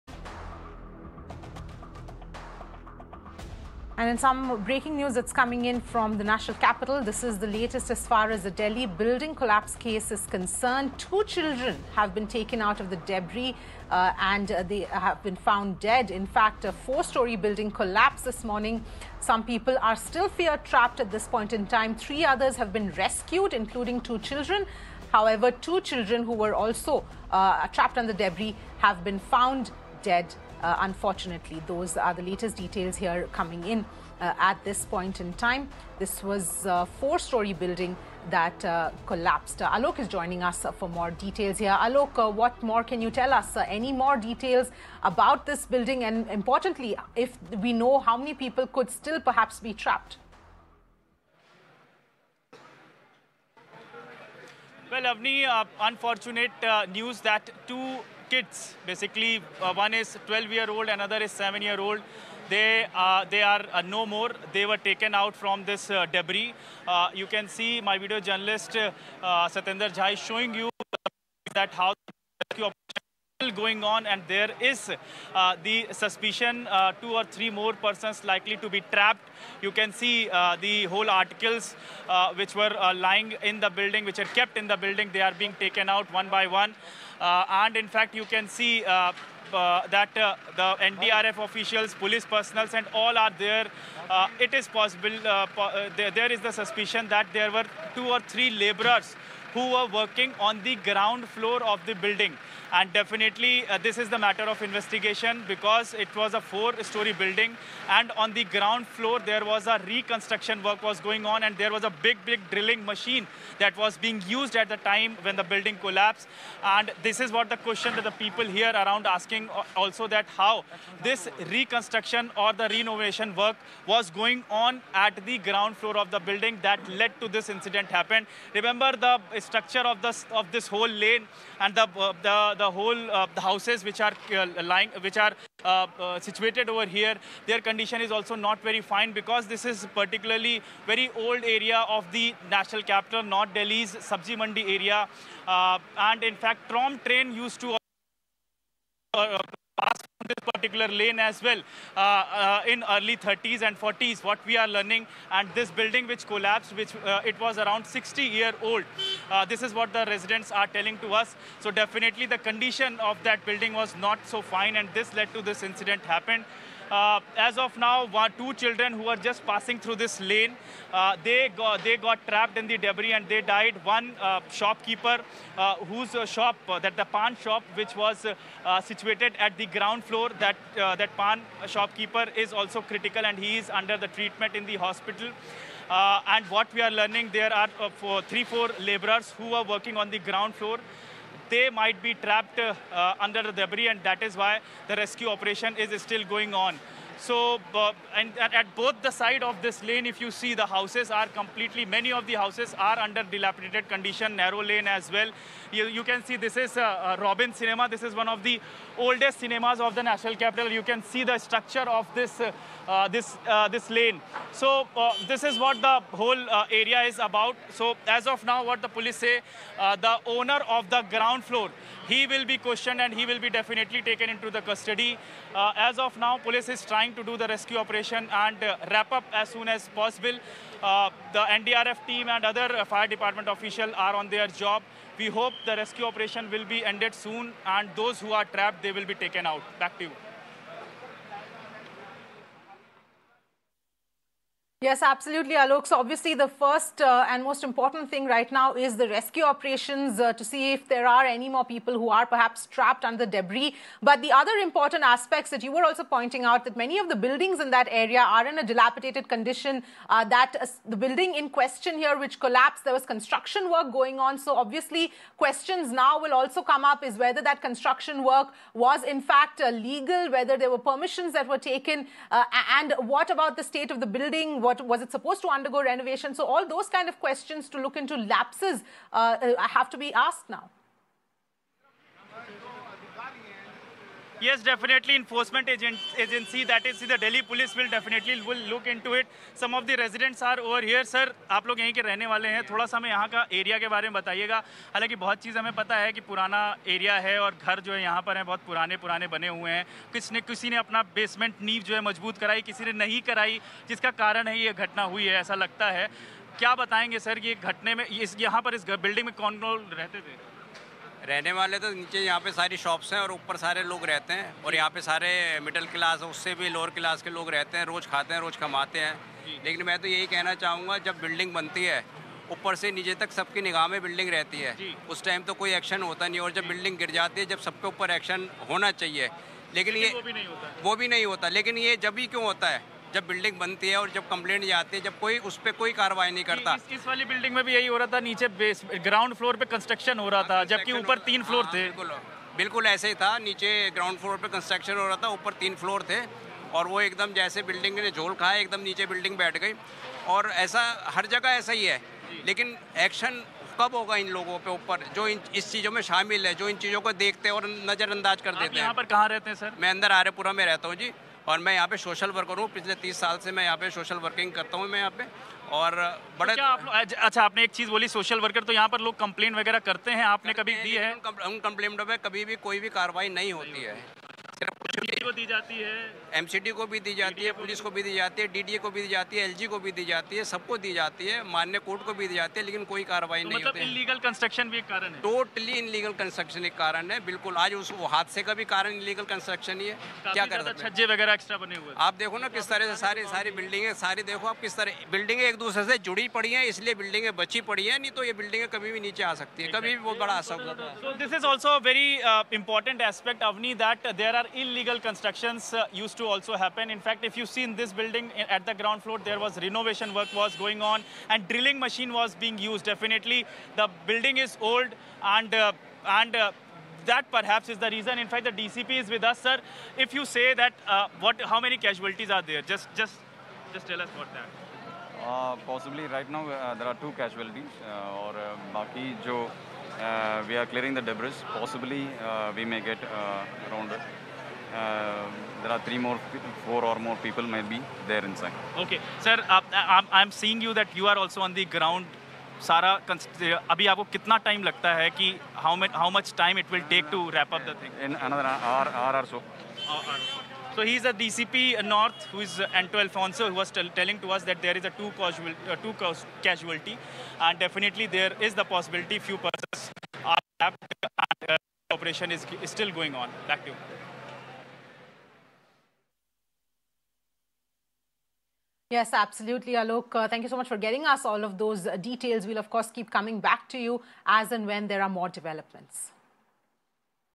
Building collapses in Sabzi Mandi area in Delhi, 2 children dead
A four-storey building collapsed in North Delhi’s Sabzi Mandi area today, leaving two children dead, and several injured. According to the police, there was a reconstruction in process in the building, and on-site, there were laborers present in the building. The rescue operations are still underway, as the Delhi Fire Service rushed to the place of the accident at 11:50 am. They believe that there are still people stuck in the debris, and the search for them is in progress.  Delhi Chief Minister Arvind Kejriwal said that he is closely monitoring the situation. He tweeted, "The incident of building collapse in Sabzi Mandi area is very sad. The administration is engaged in relief and rescue work. Through the district administration, I am myself monitoring the situation."